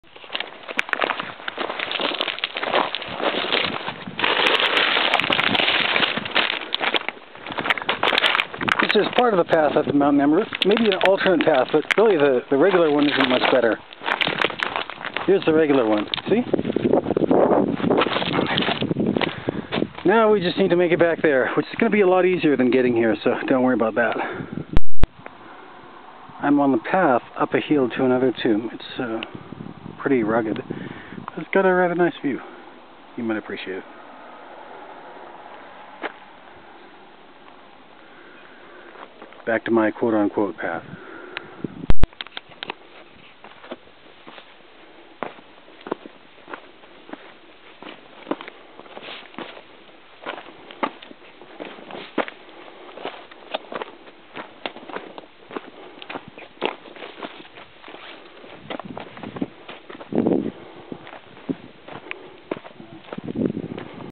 Walking on Rocks
With this video, I'm probably stretching your interest, but remember: I'm a prairie boy, and we don't have a lot of rocky stairs.

These are all from the day that we visited Mt. Nemrut, btw.